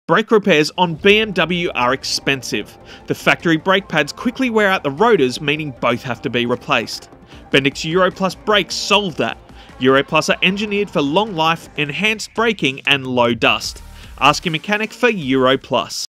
Bendix Euro+ for BMW
For a long-life, low-dust, and OE quality braking solution for your BMW, without OE prices, Bendix has you covered with the Euro+ range.